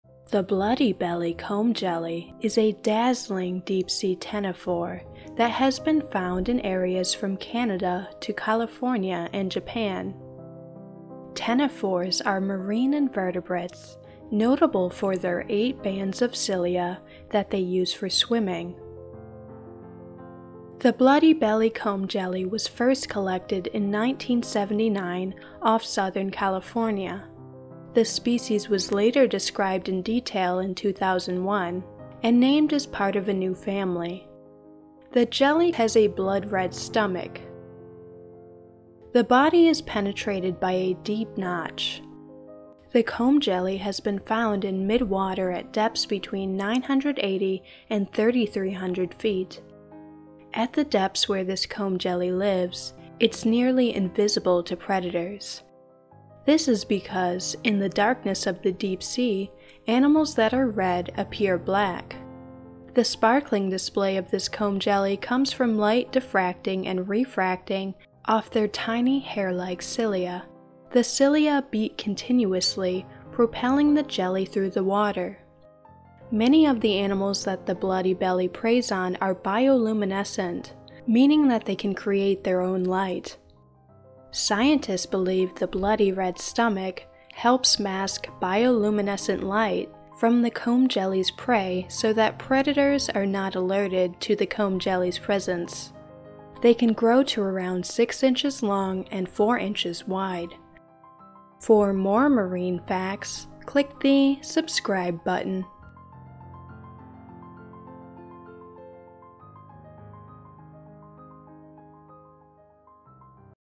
Facts: The Bloody-Belly Comb Jelly
Quick facts about this dazzling deep sea comb jelly with a bright red stomach! The bloody-belly comb jelly (samurai helmet jelly, red lobate comb jelly, Lampocteis cruentiventer). Bloody-belly comb jelly facts!